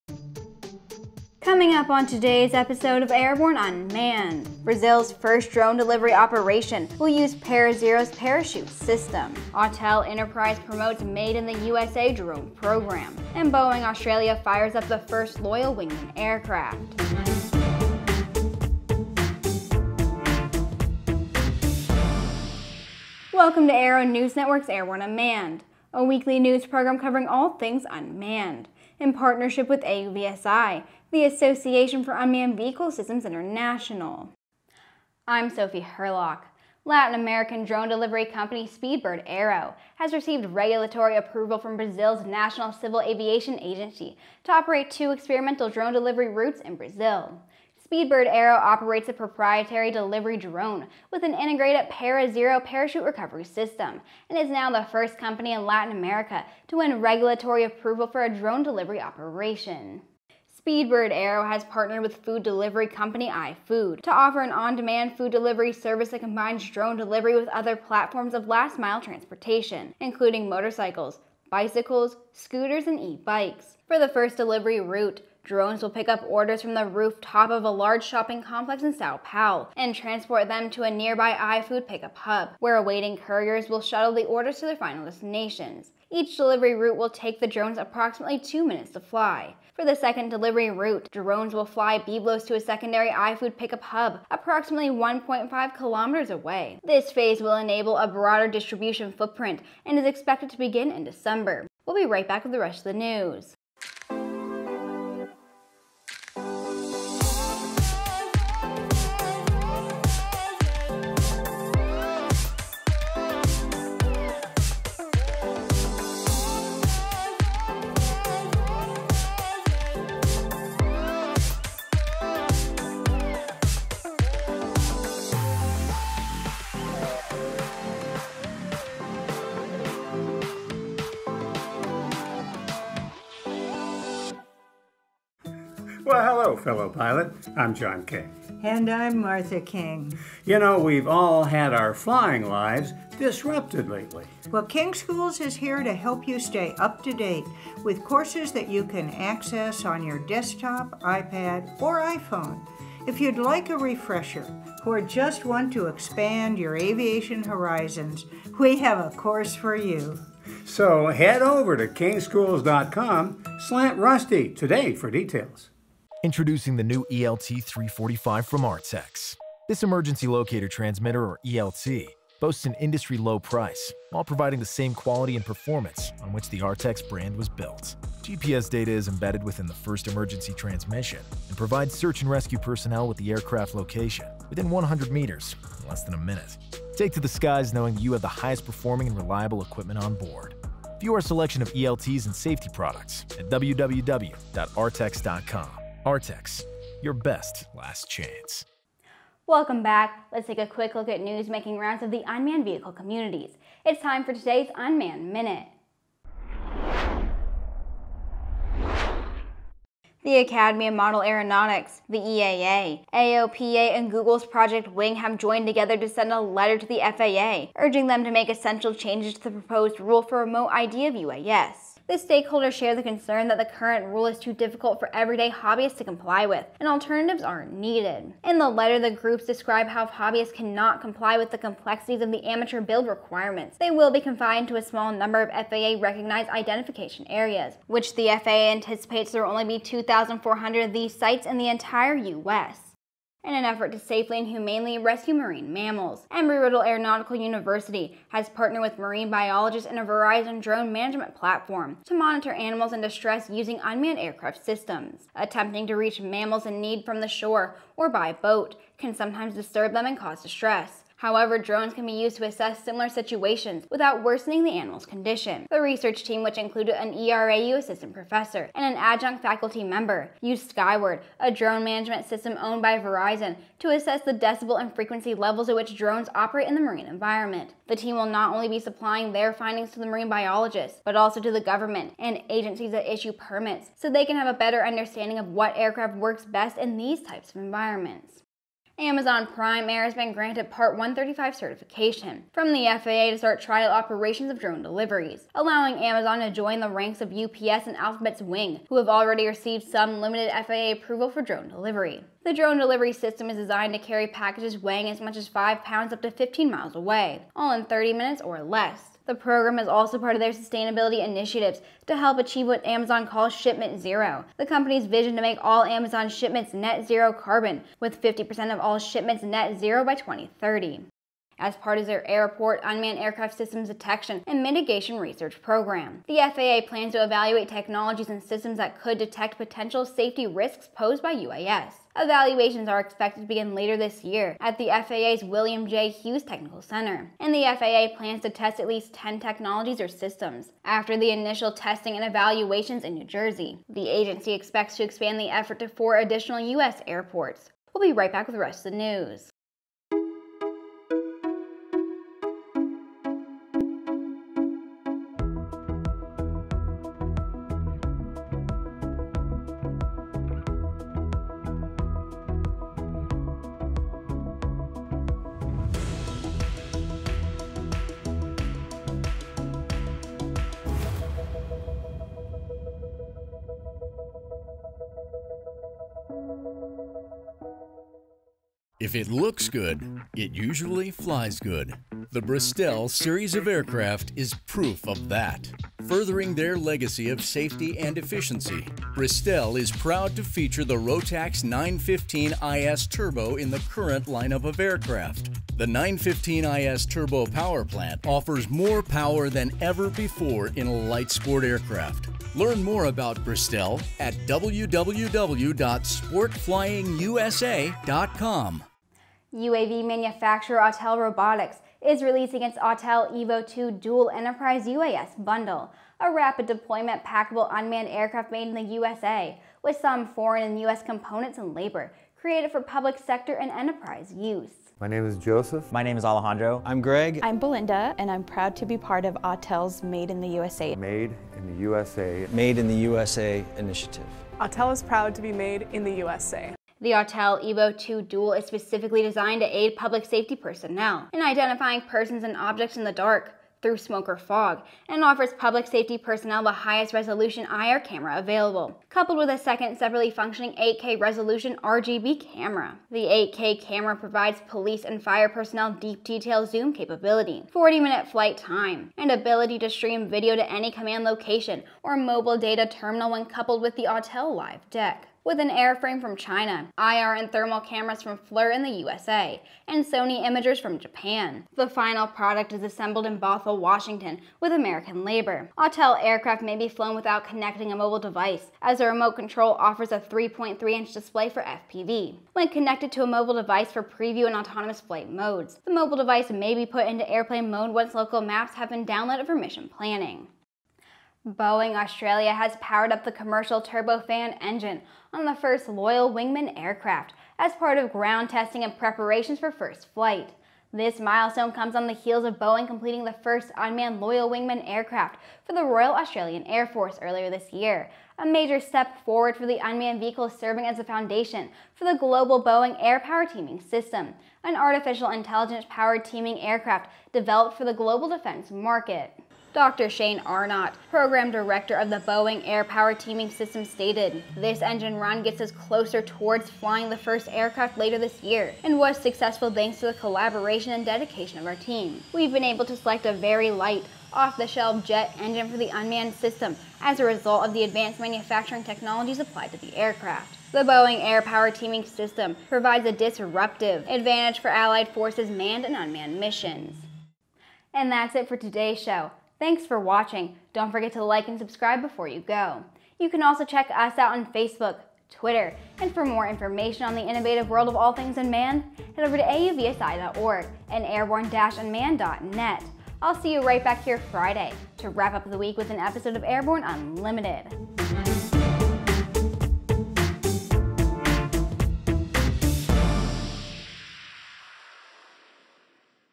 Airborne-Unmanned 09.16.20: Brazil's Drone Delivery, Autel, Loyal Wingman
Also: Remote ID Changes?, Amazon Prime Air, Detection & Mitigation, Marine Mammal Rescue

Latin American drone delivery company, Speedbird Aero, announced it has received regulatory approval from Brazil's National Civil Aviation Agency, also known as ANAC, to operate two experimental drone delivery routes in Brazil. Speedbird Aero operates a proprietary delivery drone with an integrated ParaZero parachute recovery system, and is now the first company in Latin America to win regulatory approval for a drone delivery operation. A rapid-deployment, packable unmanned aircraft made in USA with (some foreign and US components and labor) has been made available for public safety, municipal, and enterprise uses. UAV manufacturer Autel Robotics is releasing its Autel EVO II Dual Enterprise UAS bundle/package, created for public sector and enterprise use. Under the Autel Enterprise label, Autel Robotics offers end-users the opportunity to use aircraft manufactured in the USA with foreign and domestic parts and labor. Boeing Australia powered up the commercial turbofan engine on the first Loyal Wingman aircraft in September, as part of ground testing and preparations for first flight. This milestone comes on the heels of Boeing completing the first unmanned Loyal Wingman aircraft for the Royal Australian Air Force earlier this year, a major step forward for the unmanned vehicle serving as the foundation for the global Boeing Airpower Teaming System, an artificial intelligence-powered teaming aircraft developed for the global defense market. All this -- and MORE in today's episode of Airborne-Unmanned!!!

Presented by Aero-TV veteran videographer and Airborne Host Sophie Hurlock and is supported by ANN CEO/Editor-In-Chief Jim Campbell and Chief Videographer Nathan Cremisino.